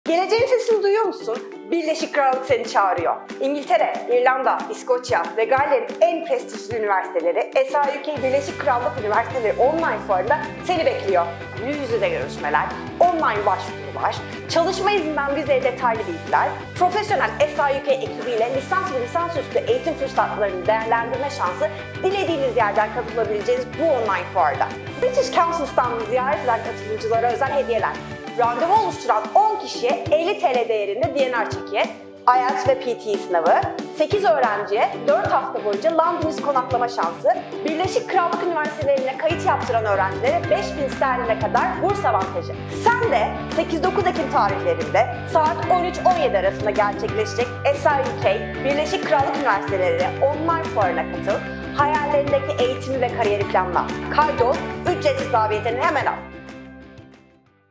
Birleşik Krallık Seni Çağırıyor!
İngiltere, İskoçya, İrlanda ve Galler’in en prestijli üniversiteleri, SI-UK Birleşik Krallık Üniversiteleri Fuarı’nda seni bekliyor!

Üstelik fuara özel hediyeler kazanma fırsatın da var!

British Council standında;
*50 TL değerinde D&R hediye çeki,
*IELS ve PTE sınavı,
*Londonist’te konaklama şansı,
Birleşik Krallık üniversitelerine kayıt yaptıranlara;
*5000GP’ye kadar burs avantajı SI-UK Birleşik Krallık Üniversiteleri Fuarı’nda.

8-9 Ekim tarihlerinde, 13:00-17:00 saatleri arasında gerçekleşecek online fuar için, siukfair.endlessfairs.com üzerinden ücretsiz davetiyeni al, seçkin eğitim kurumlarını yakından tanı, fırsatları yakala!